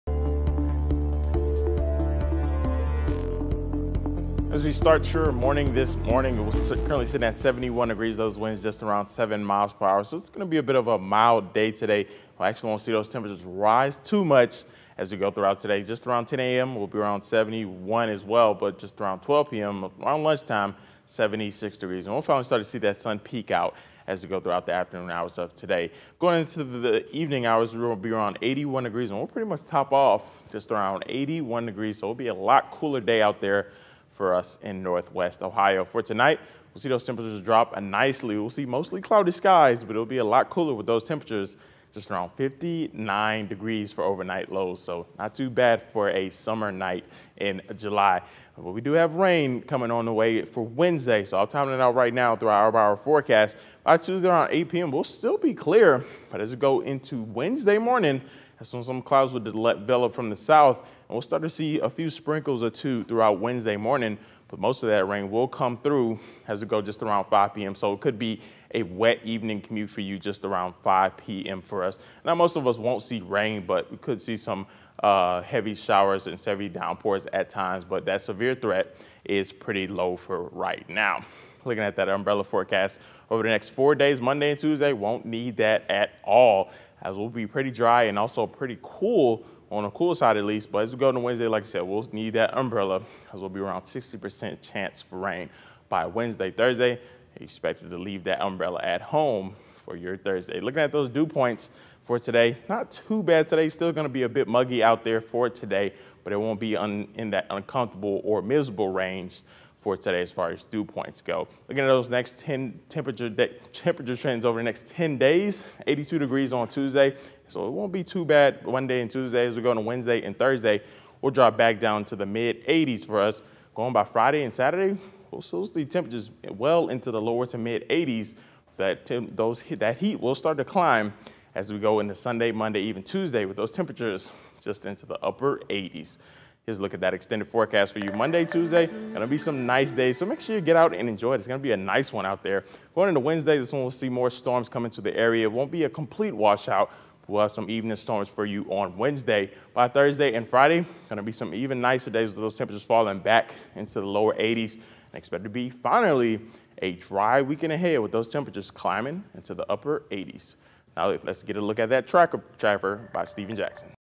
Dry today, temps in the low 80s
A break from the heat with temps in the low 80s today and tomorrow; dry Monday and Tuesday with a chance of rain on Wednesday.